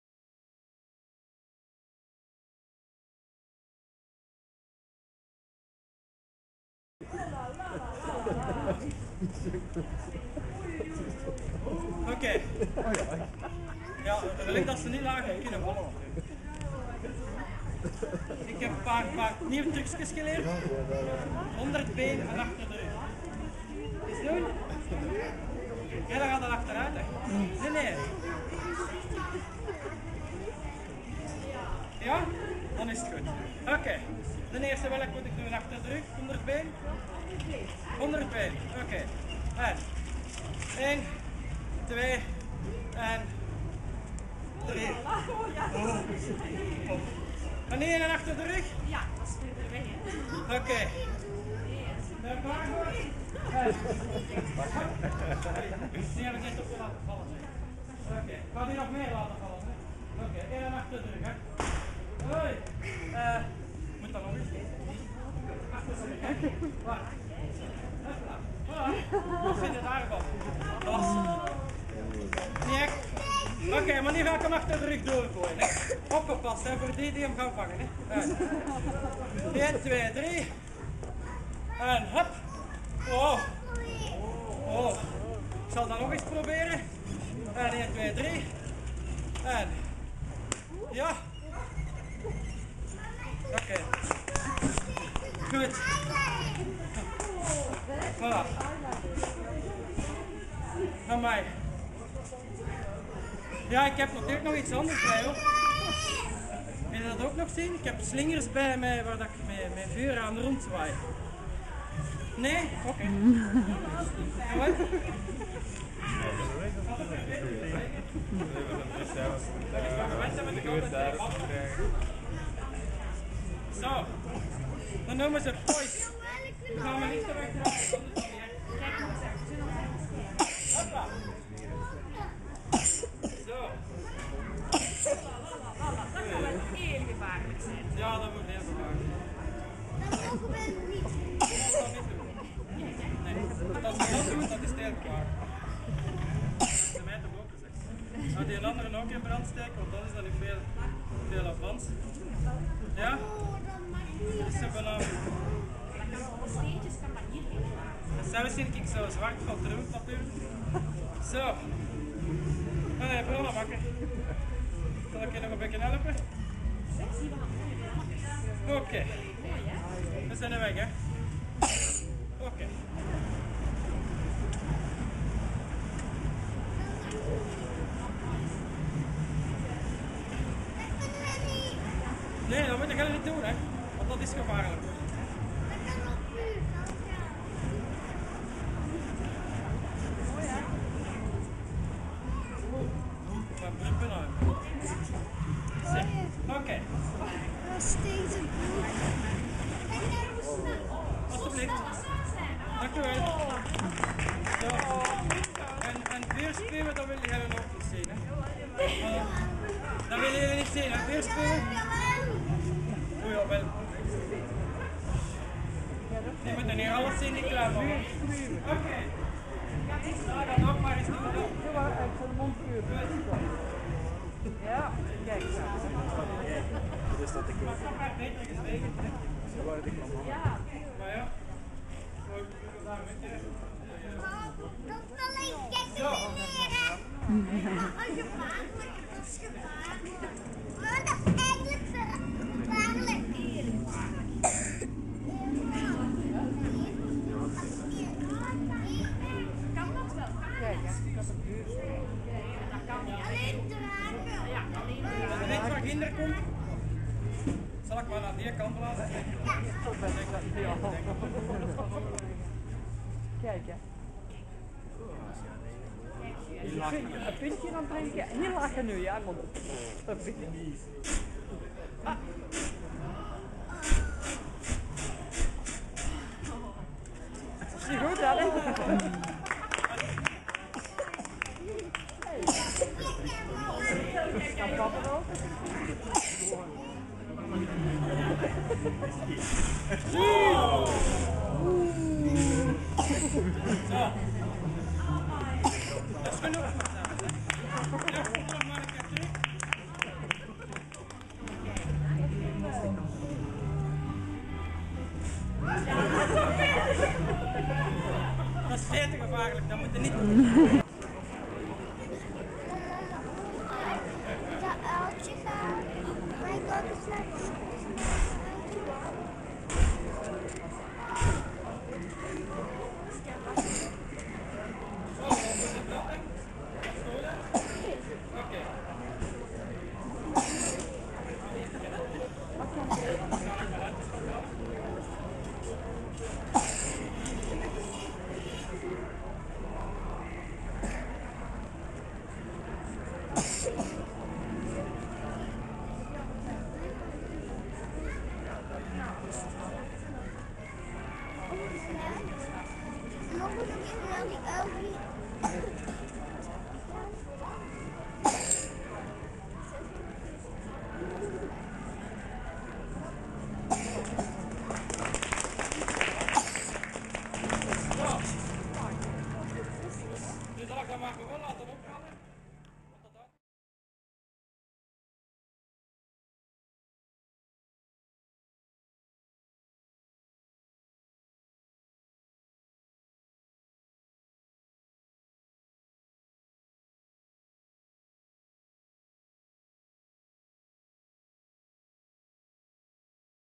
Halloween fire show for Studio Triskel
Fire breaing, juggling and humor for the Halloween street event in Tienen (belgium)